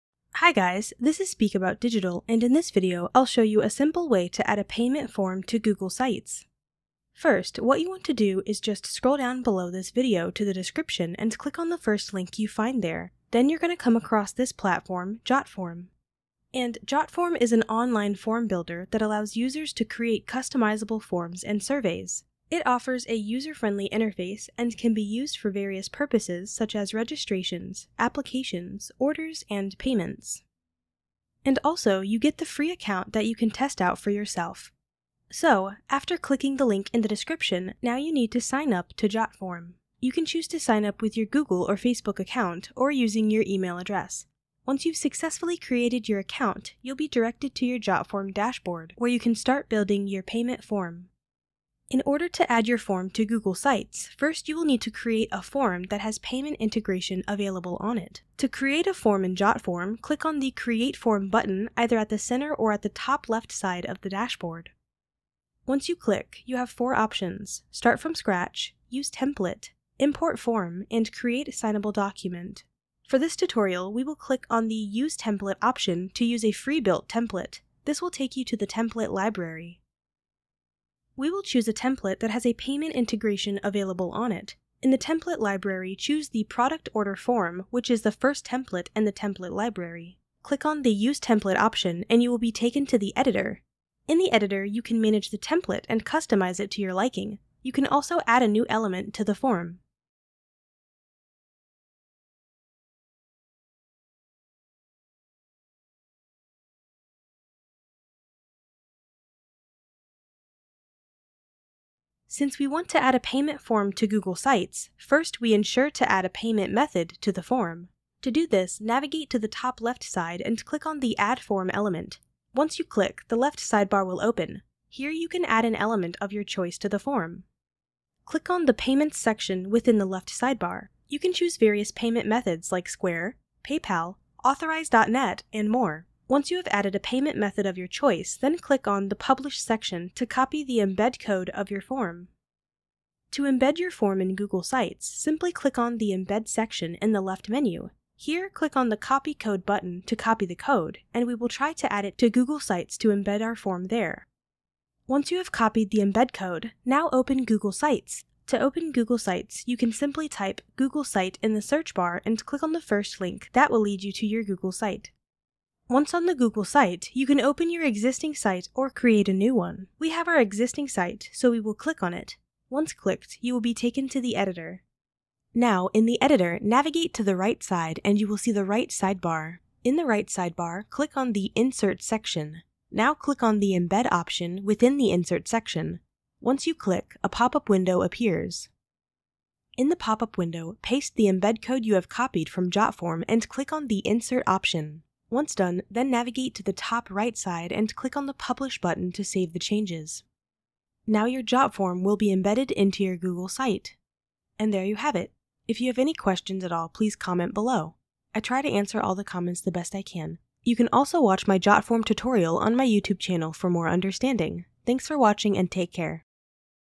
How to add a payment form to Google Sites
This is my video on How to add a payment form to Google Sites for beginners. I cover the most basic functions.
It's really easy to learn how to do it in just a few minutes by following this super helpful tutorial.